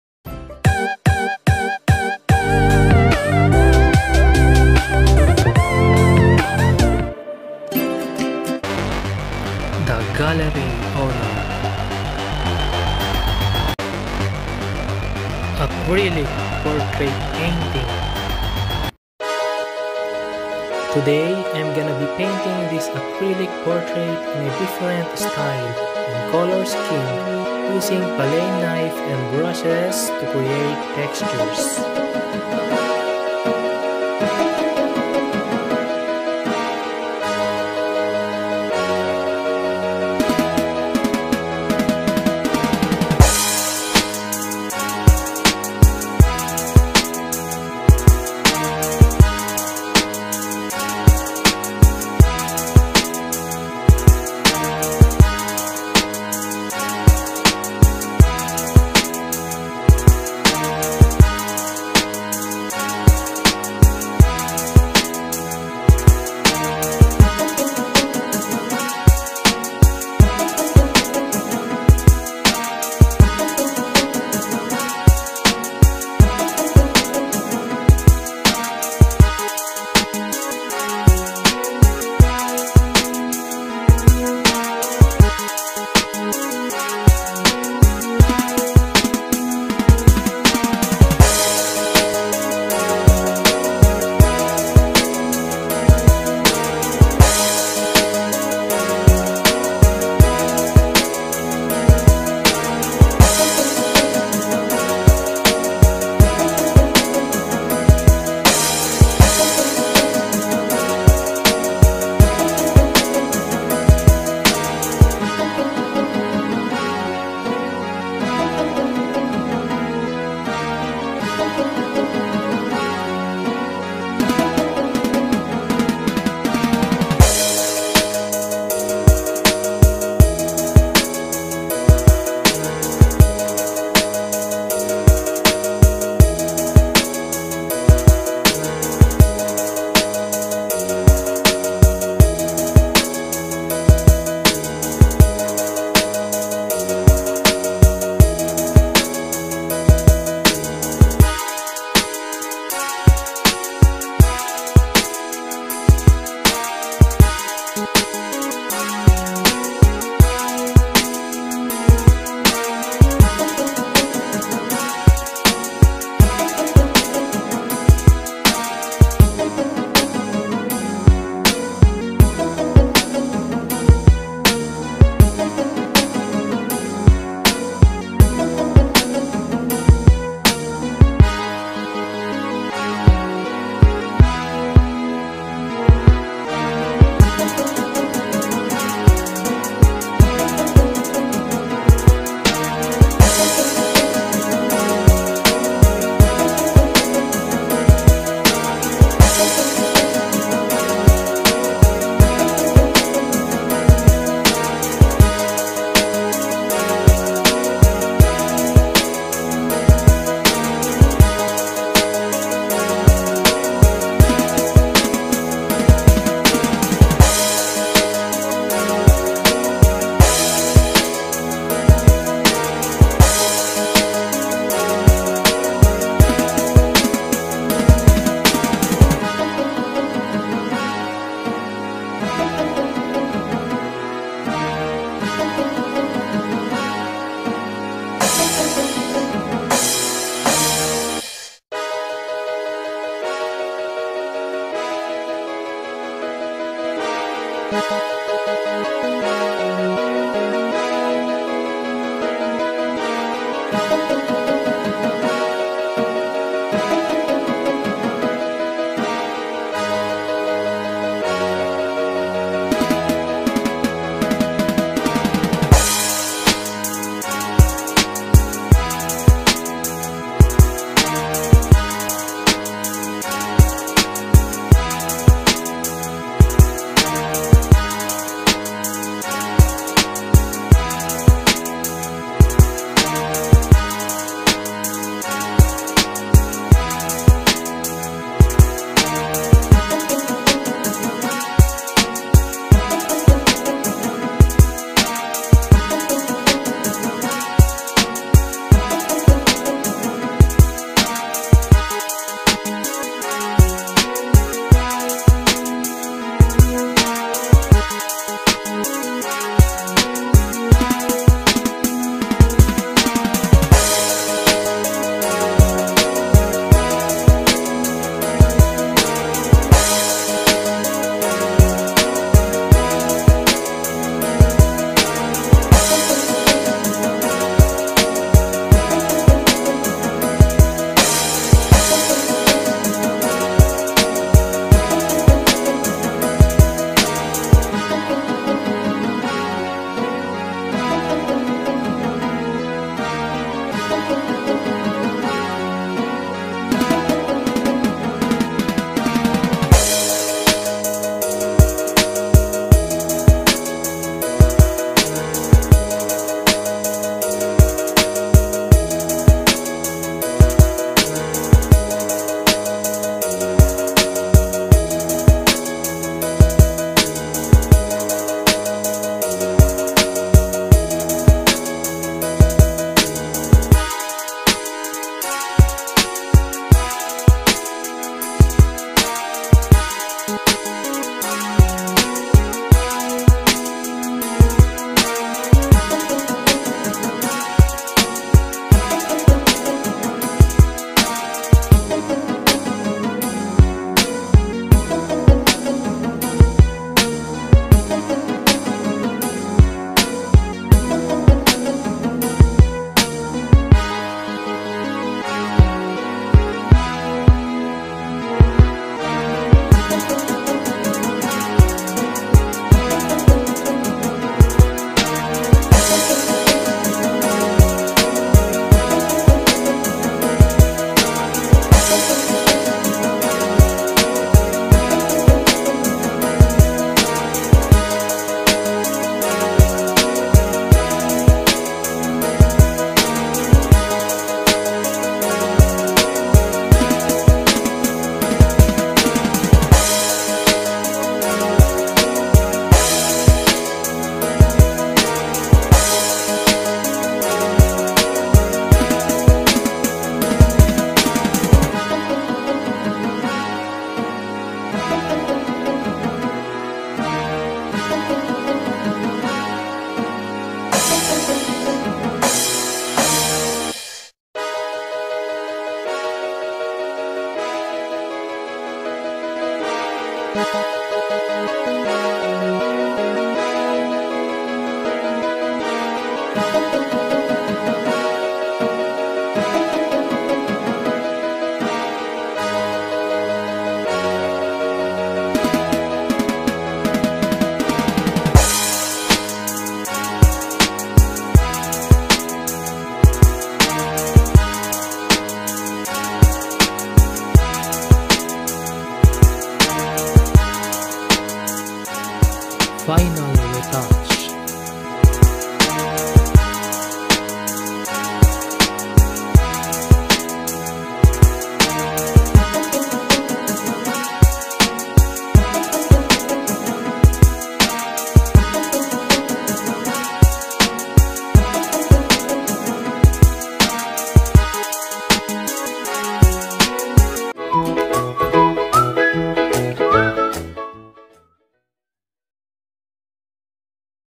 PORTRAIT PALETTE KNIFE PAINTING
Title: PORTRAIT PALETTE KNIFE PAINTING

I made this abstract oil portrait palette knife painting of a good friend the gallery owner, Sir Matti Sirvio as a gift for him. I recorded this time-lapse painting video on how I created those textured abstract brush strokes and palette knife strokes.
In this video I show how I paint his oil portrait in step by step process.